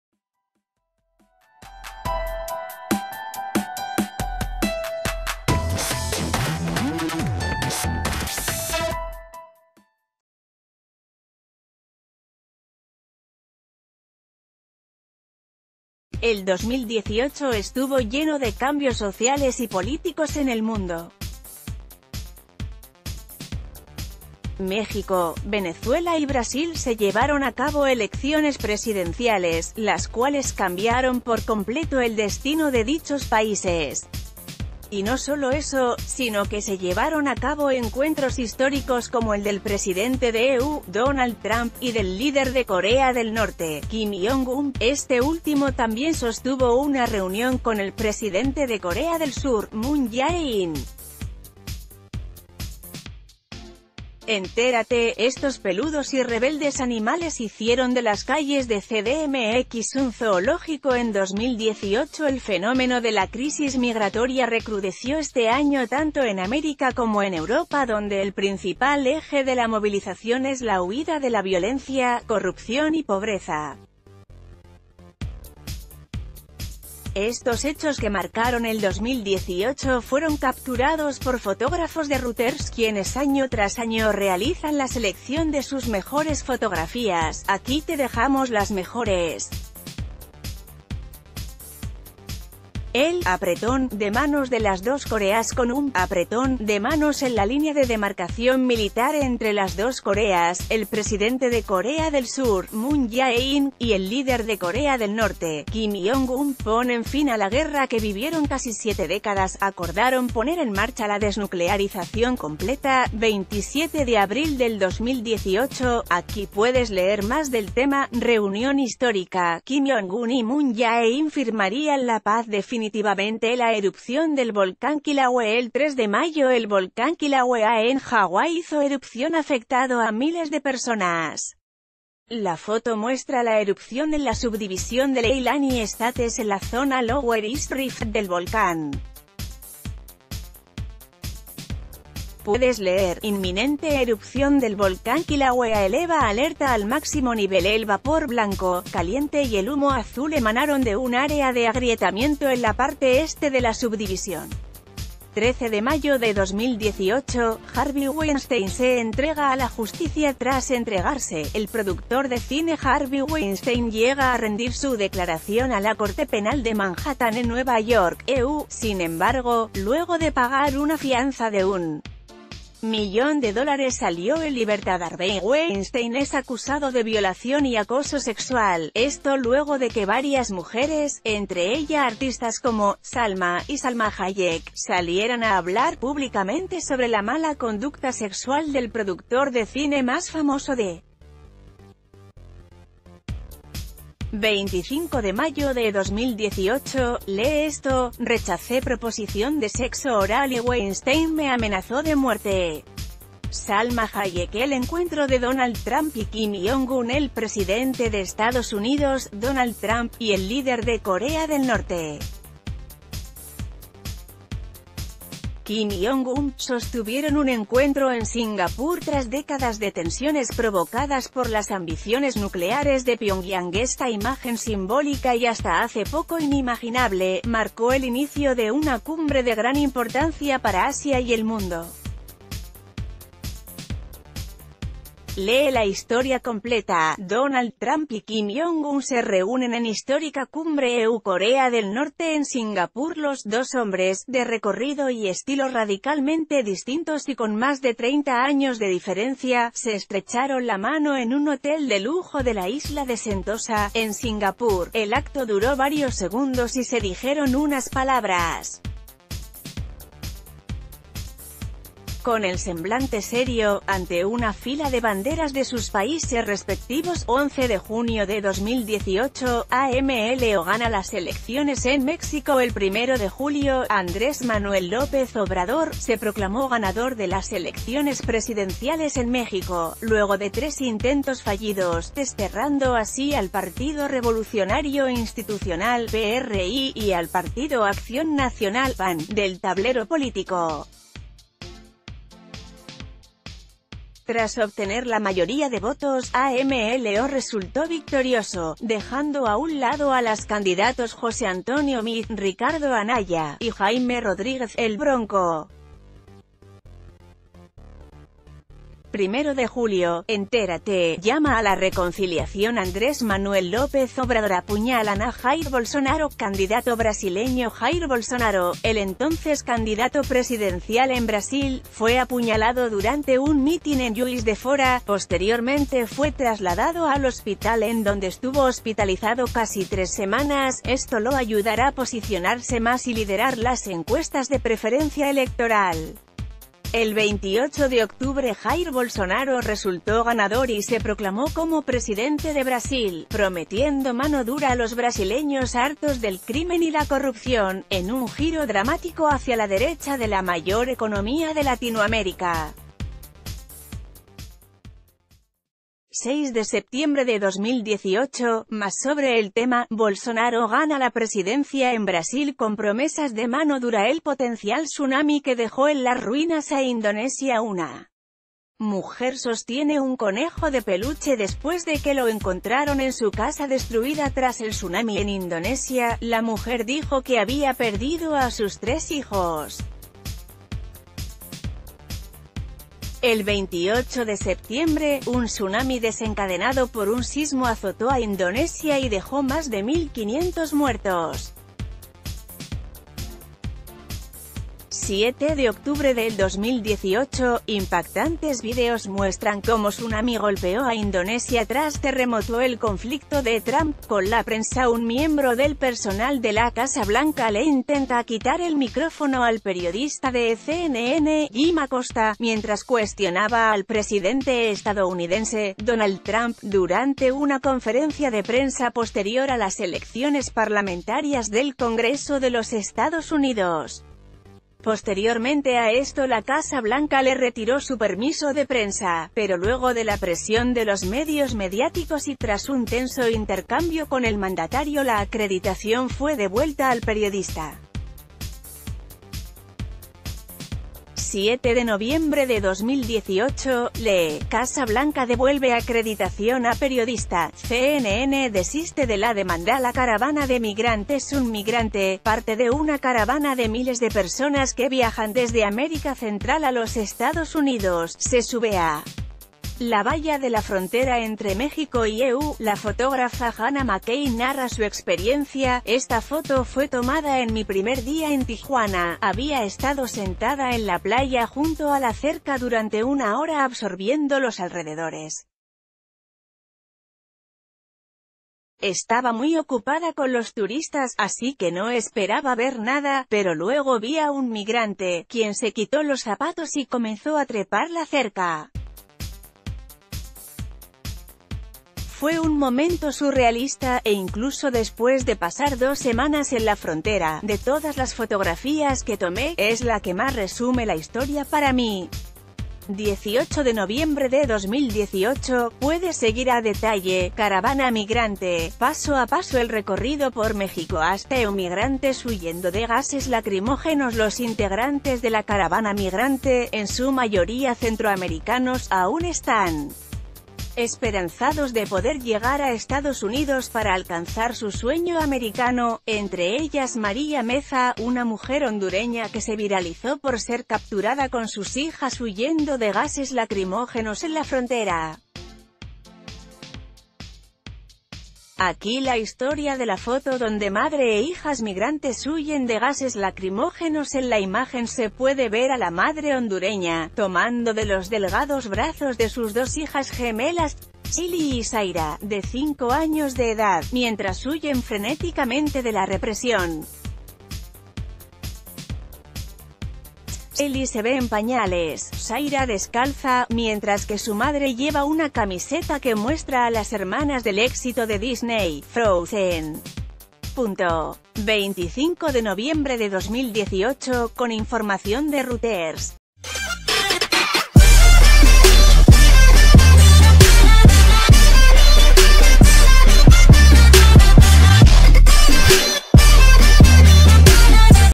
Estas son las fotos que marcaron la historia del 2018
El 2018 estuvo lleno de cambios sociales y políticos en el mundo. México, Venezuela y Brasil se llevaron a cabo elecciones presidenciales, las cuales cambiaron por completo el destino de dichos países. Y no sólo eso, sino que se llevaron a cabo encuentros históricos como el del presidente de EU, Don...